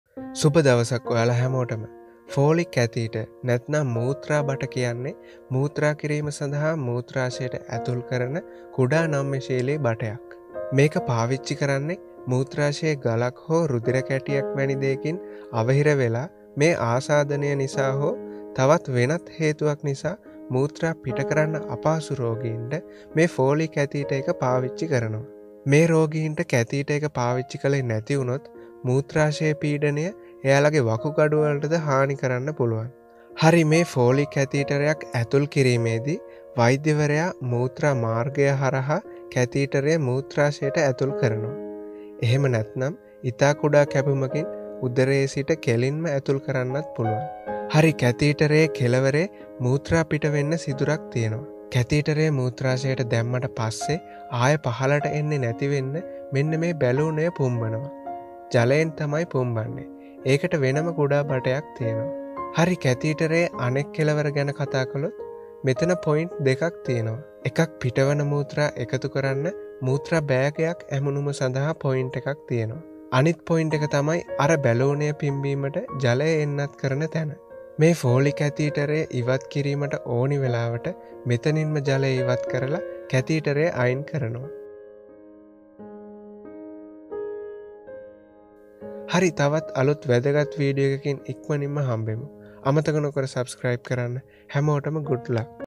මූත්‍රා බට 😨| Foley catheter , its insertion and function in sinhala explanation
ෆෝලි කැතීටරයක් ​​රෝගියාගේ මුත්‍රාශයට දැමීමෙන් මුත්‍රා අඛණ්ඩව බැස යාමටත් මුත්‍රාශය හිස්ව තබා ගැනීමටත් ඉඩ සලසයි. මෙය මුත්රාශය, මුත්රා සහ වකුගඩු ආරක්ෂා කිරීමට උපකාරී වේ.

Urine can continuously drain and keep the bladder empty when a Foley catheter is inserted into the patient's bladder. This aids in kidney, ureter, and bladder protection.